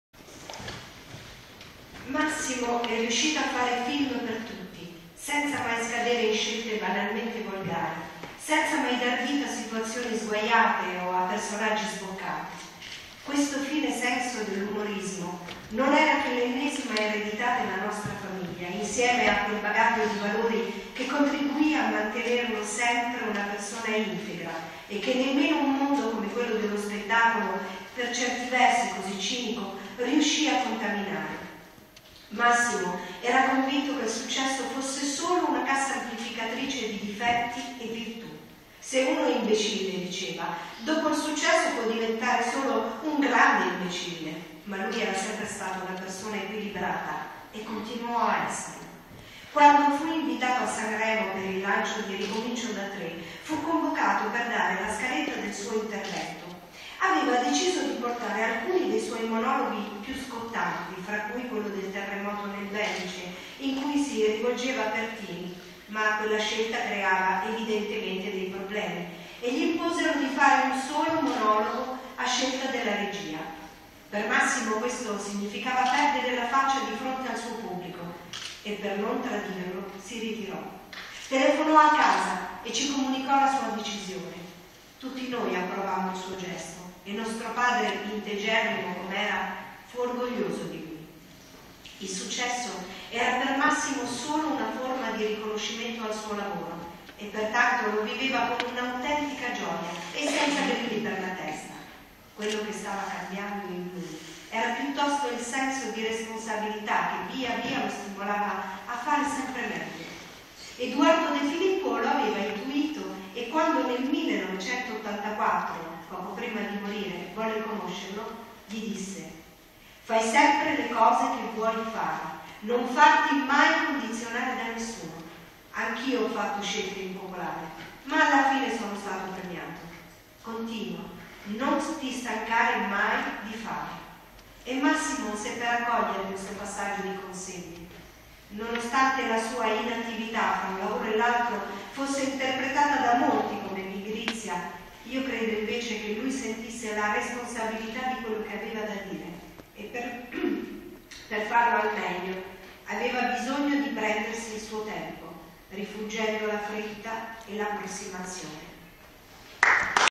L'attrice comica e cantante della Tuscia Laura Leo legge un brano del libro sorella Troisi
L'attrice comica e cantante della Tuscia Laura Leo legge un brano del libro sorella Troisi, Rosaria, dal titolo "Oltre il respiro. Massimo Troisi, mio fratello".